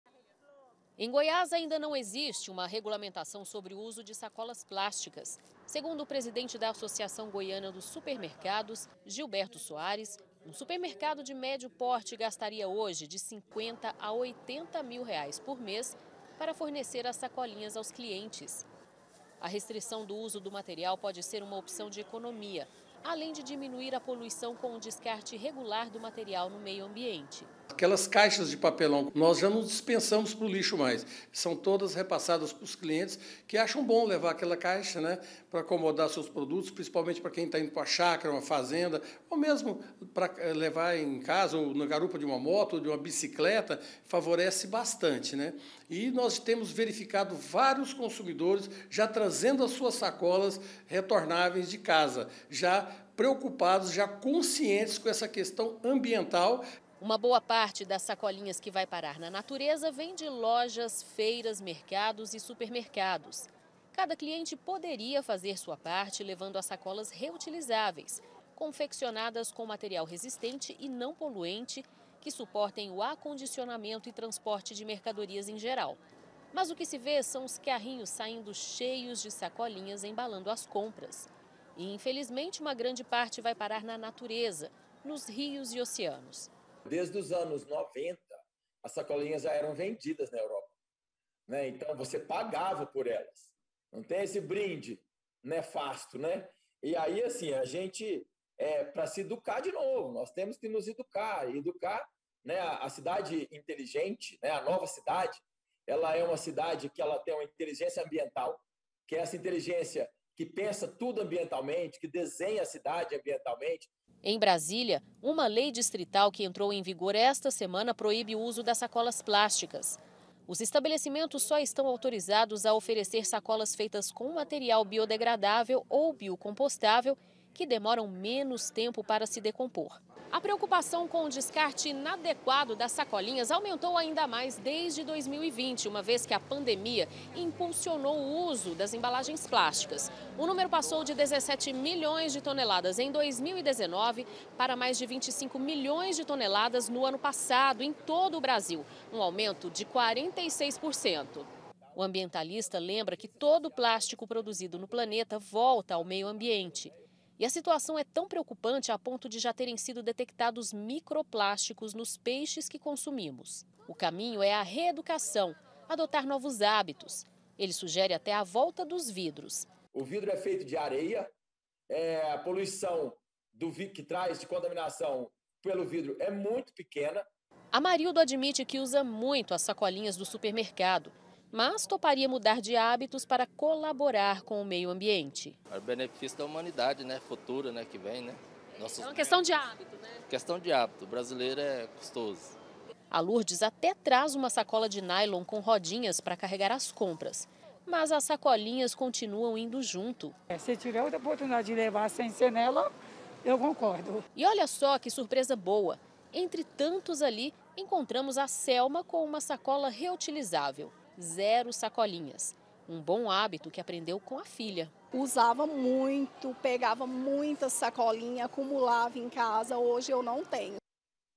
SACOLINHAS PLÁSTICAS: USO EM EXCESSO PREJUDICA O MEIO AMBIENTE
DAS 27 CAPITAIS BRASILEIRAS, 17 JÁ PROIBIRAM AS SACOLAS PLÁSTICAS PRA REDUZIR O IMPACTO AO MEIO AMBIENTE. A ÚLTIMA FOI BRASÍLIA. A LEI POR LÁ PASSOU A VALER ESTA SEMANA. EM GOIÁS, NÃO HÁ NEM DISCUSSÃO SOBRE O ASSUNTO. MAS SUPERMERCADOS E AMBIENTALISTAS DIZEM QUE É PRECISO, NO MÍNIMO, REDUZIR O USO DA SACOLINHA.